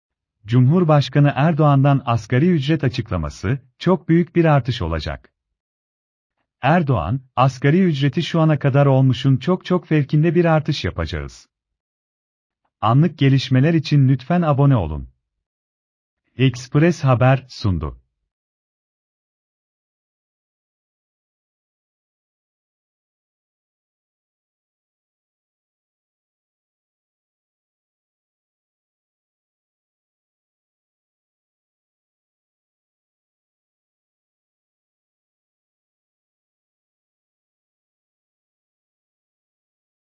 SON DAKİKA: Cumhurbaşkanı Erdoğan’dan asgari ücret açıklaması: Çok büyük bir artış olacak
Erdoğan: Asgari ücreti şuana kadar olmuşun çok çok fevkinde bir artış yapacağız.